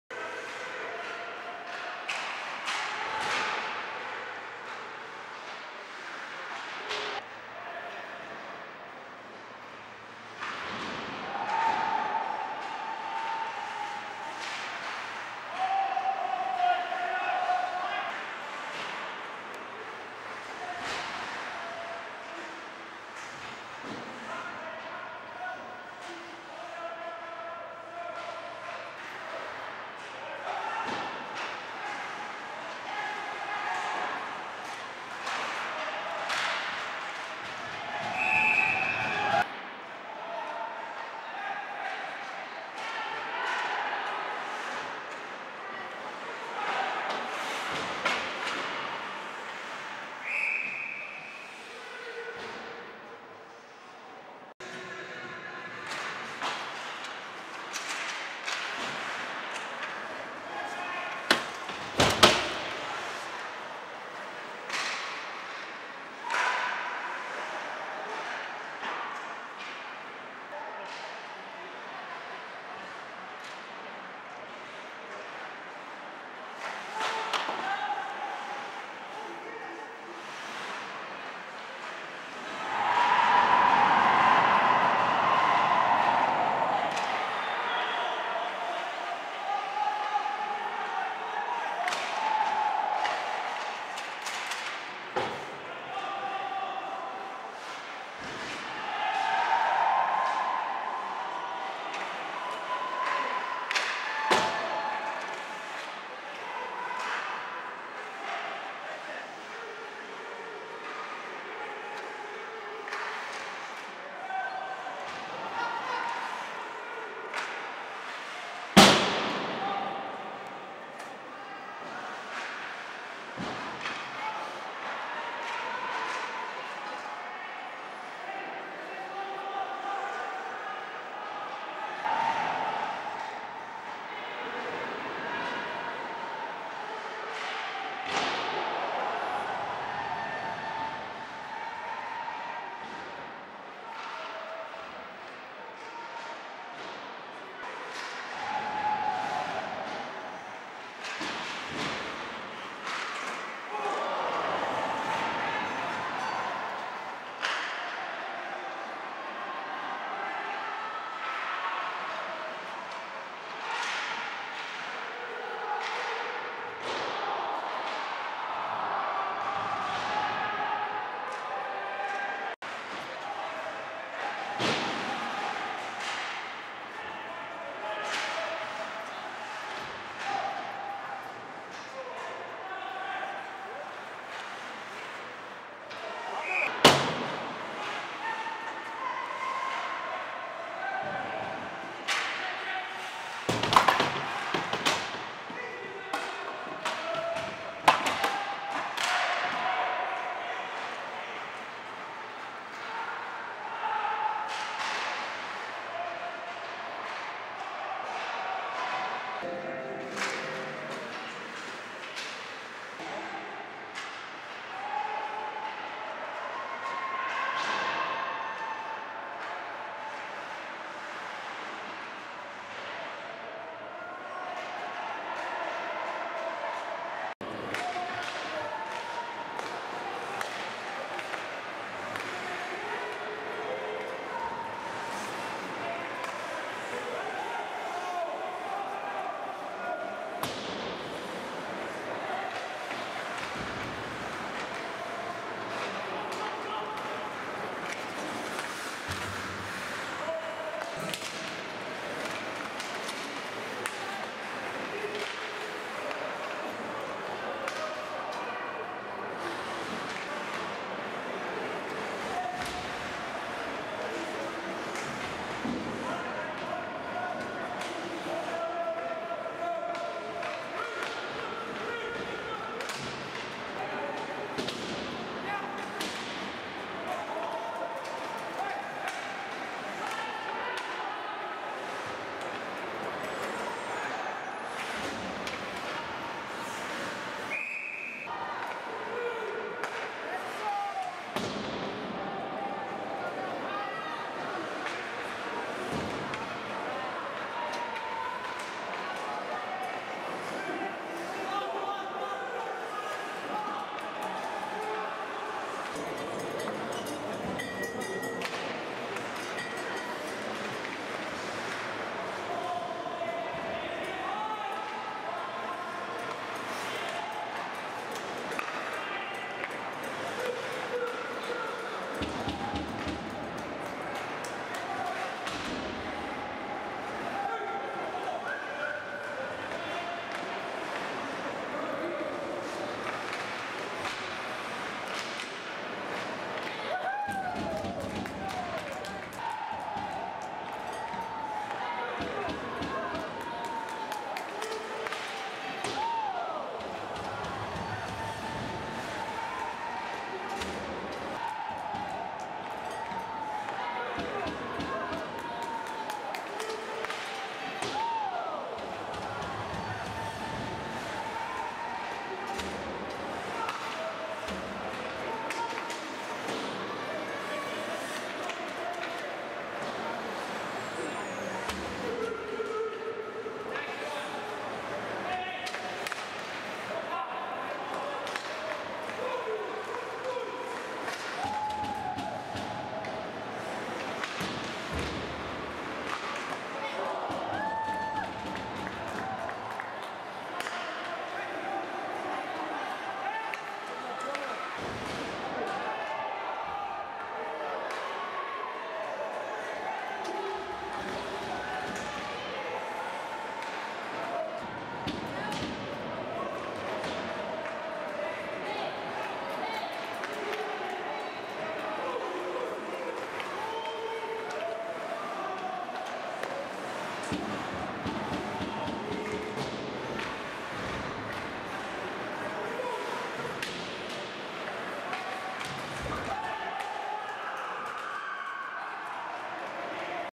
Nikiforuk Game Footage vs North Okanagan Knights Oct 14, 2017 KIJHL
Colton Nikiforuk #10 Chase Heat Black KIJHL all shifts.